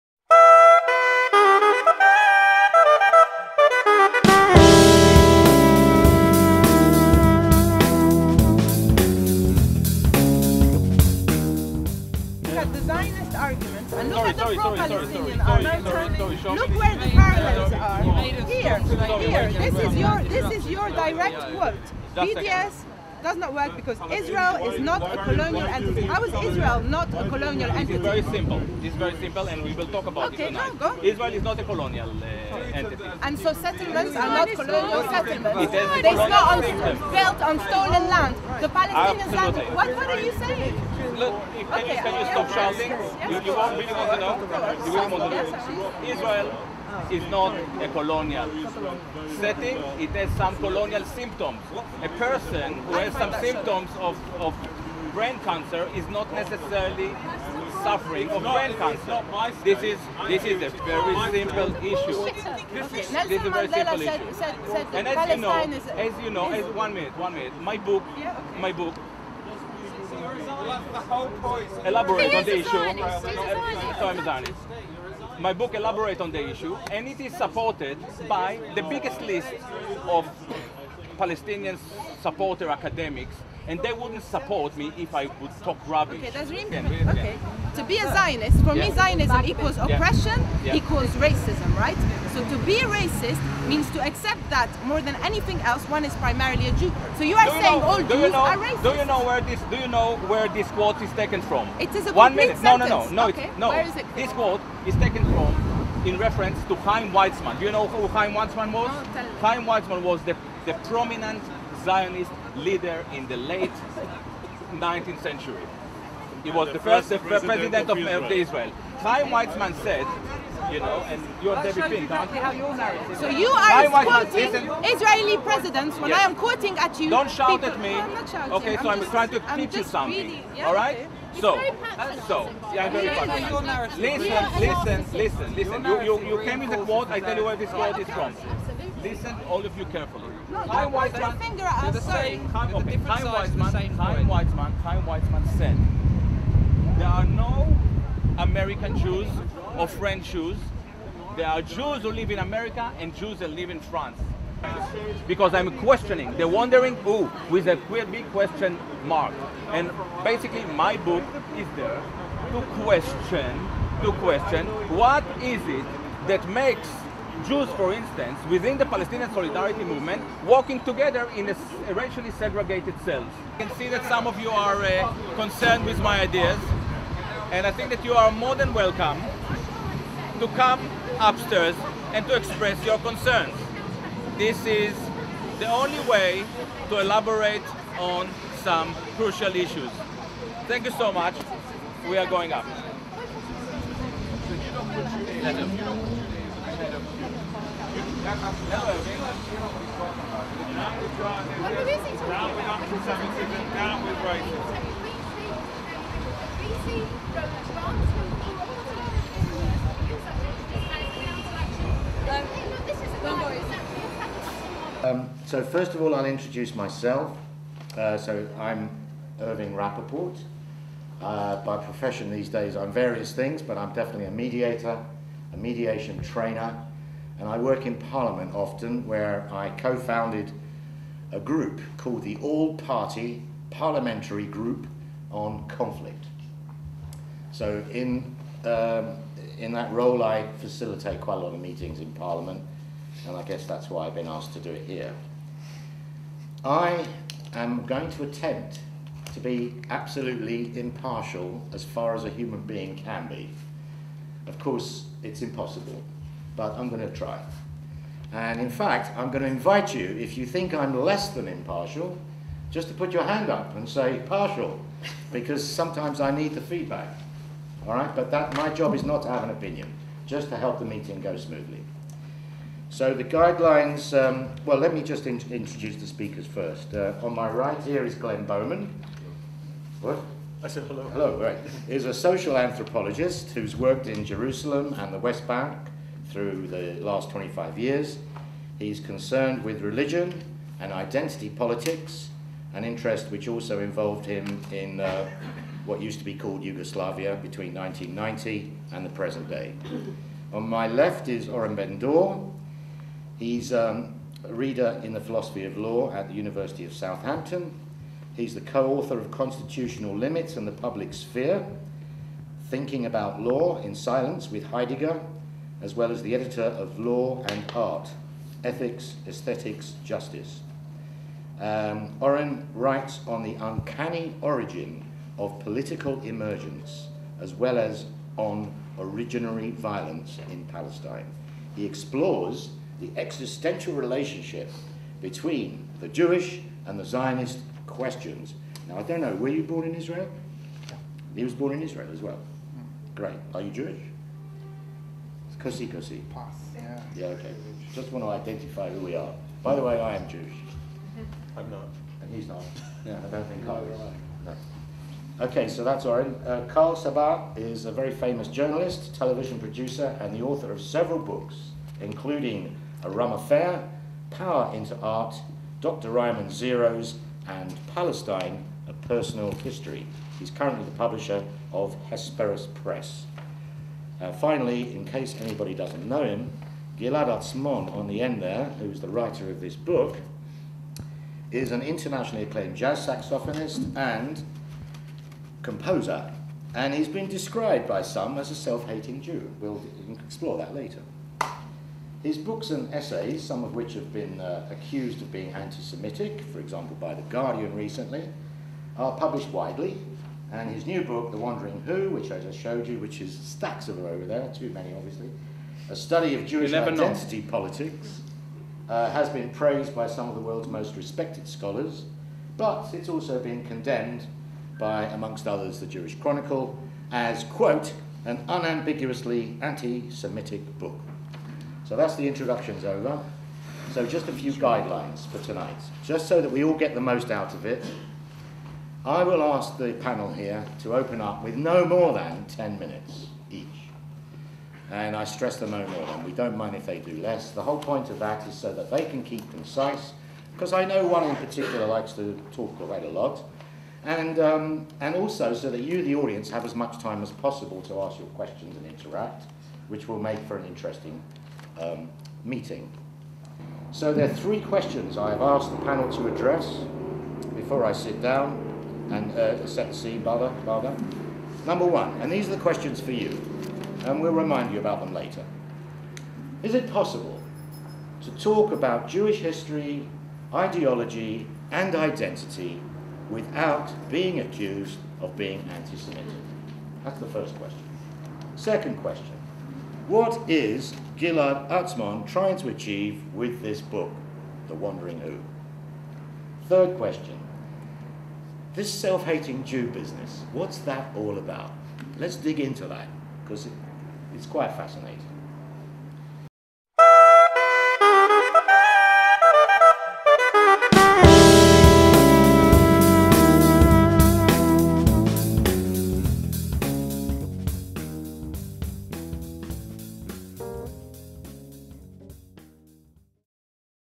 Panel Discussion on 'Jewish Identity Politics' Part 1
Part 1 contains some footage from a picket organised by J-BIG  that took place shortly prior to Gilad Atzmon's 'The Wandering Who' book launching event and Panel Discussion
 Panelists:  Glenn Bowman, Karl Sabbagh, Oren Ben-Dor and Gilad Atzmon
Facilitator: Irving Rappaport